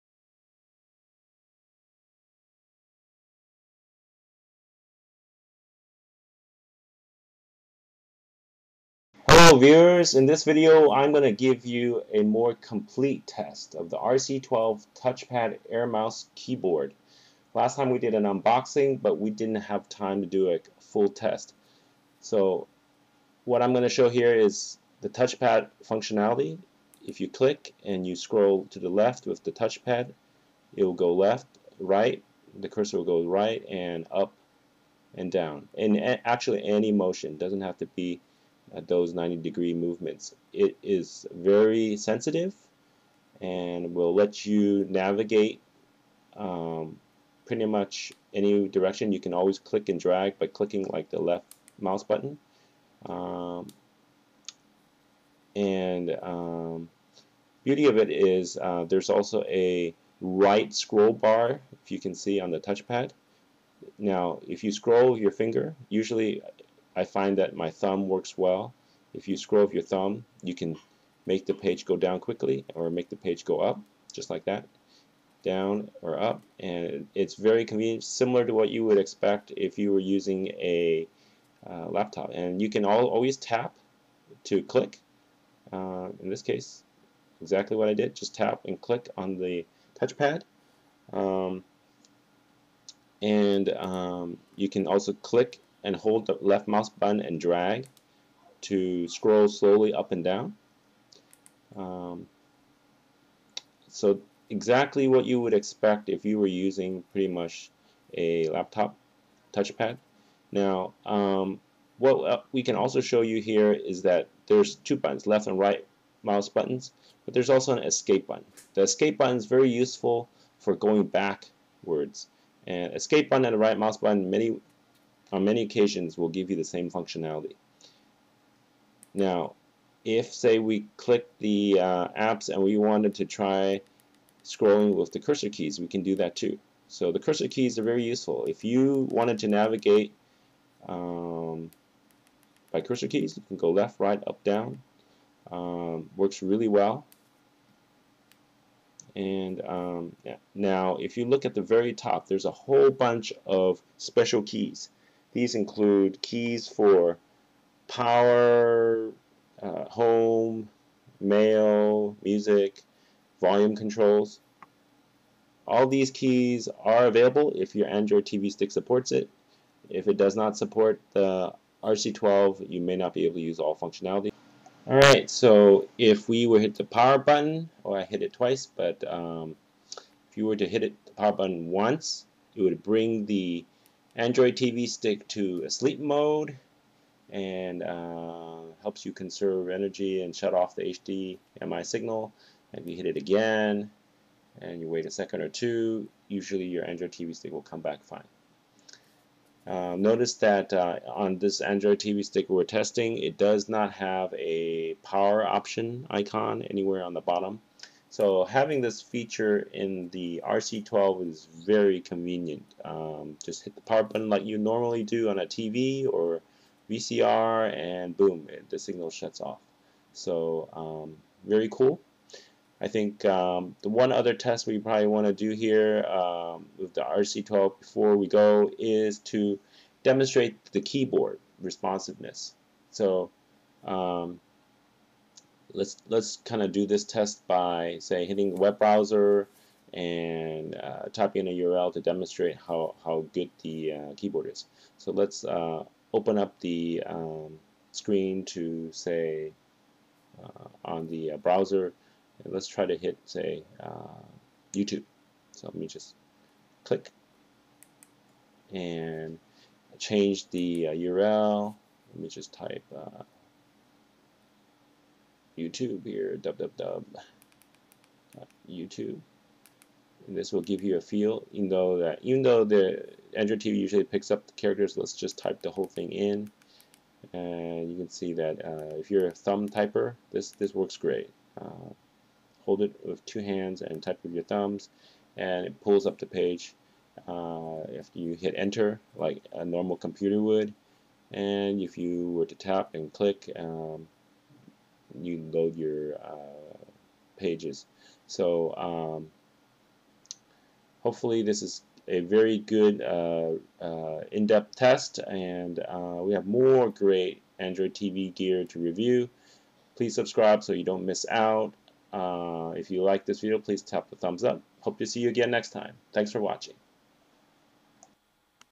Measy RC12 Air Mouse Keyboard Touchpad Unboxing & Test - Part 2
Based on your comments and requests, we have added a Part 2 which goes in-depth with a look at your experience when using the controller on an Google Android TV box.  We cover the use of the touch pad, cursor keys, and important function keys such as the power button to enable sleep mode on the TV box.

If you want to see more of the RC12 in action, please look at our unboxing and test of the GT-S21D Android TV stick.  We use the RC12 exclusively in our Google TV review.

You may also want to compare to the RC13 Voice Air Mouse Keyboard